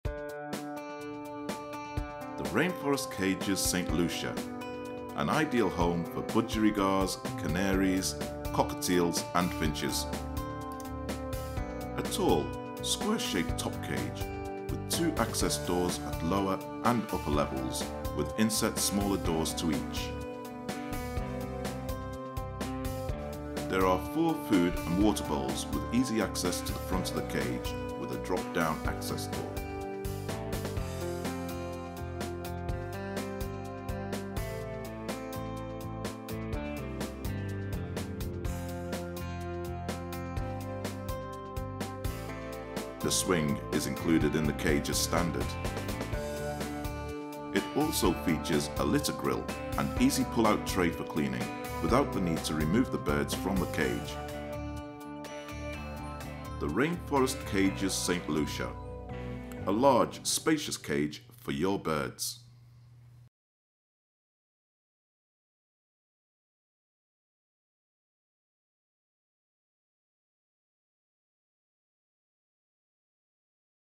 Small Bird Cage - St Lucia - Sky Pet Products
This elegant large bird cage is perfect for any small bird or multiple group of birds This cage is ideal for Budgies, Cockatiels, Lovebirds, Parakeets, Small Conures, Finches and Canaries.
The cage features 3 wooden perches, 4 feeder dishes, 2 large front opening doors, a swing, a pull out cleaning tray and safety grill and a convenient carry handle.
The cage can be used in conjunction with out Rainforest Columbia Bird Cage Stand as per the secondary images.
Measurements:
W46 x D36 x H92cm with 1cm bar spacing.
Black Order Code: 4876
White Order Code: 4886